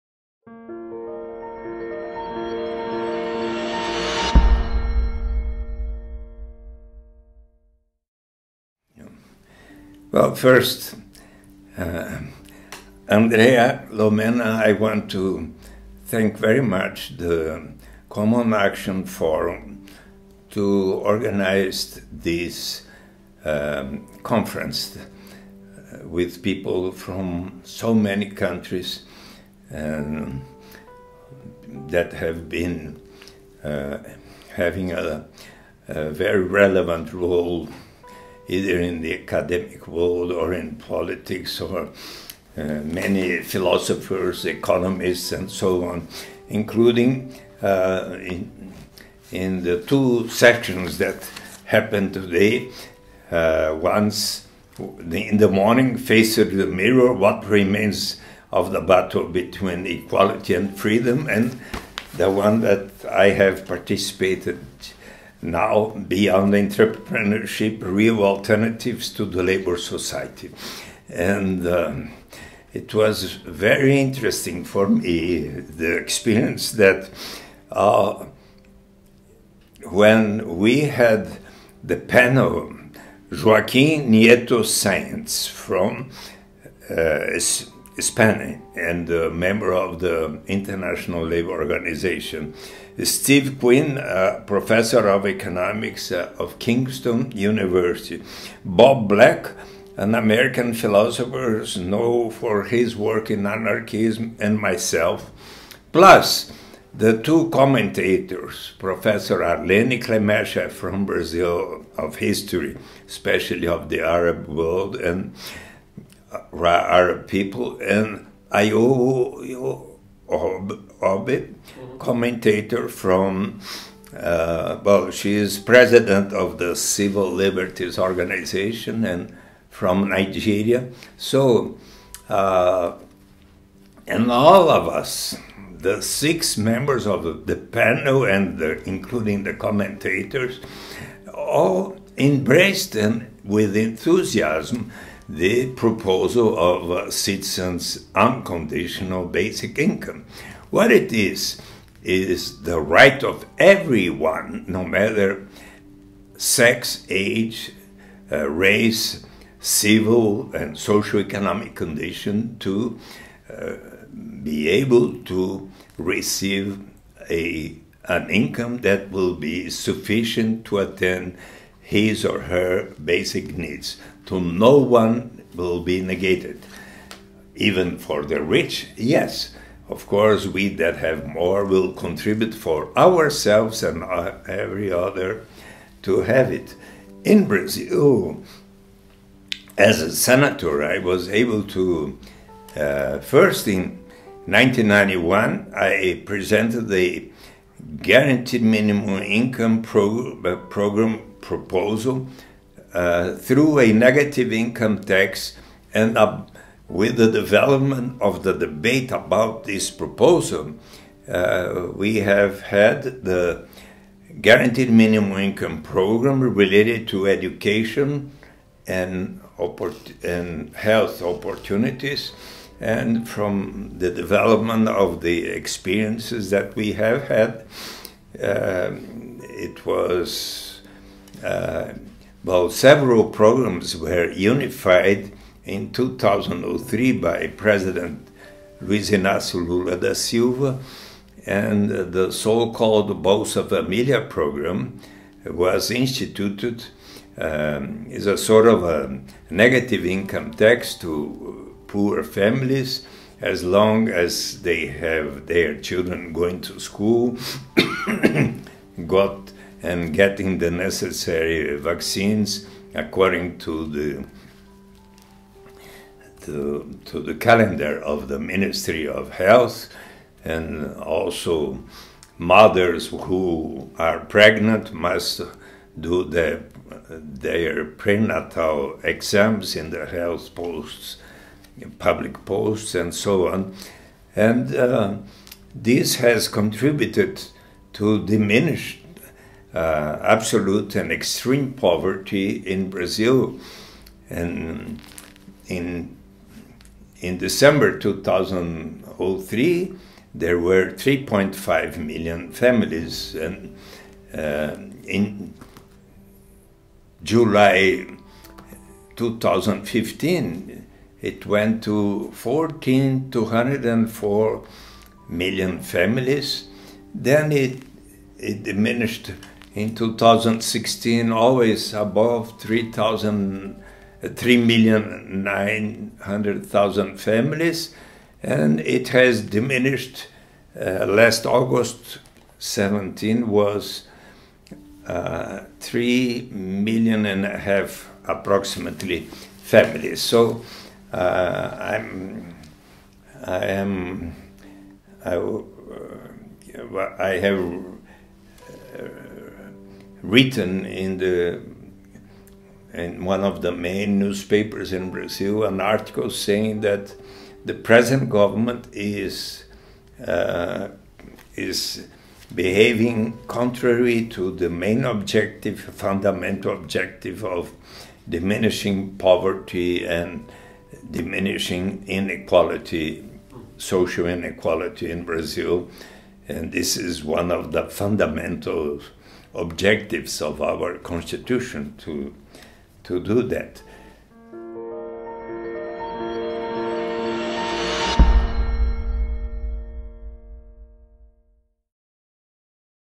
CAF2017 Interview - Eduardo Suplicy (I)
Interview with Eduardo Suplicy, Brazilian professor, economist and politician, at CAF2017 annual Forum that took place in Madrid, from November 3rd to 5th, under the title 'The Limits to Inequality: seeking a sustainable society'.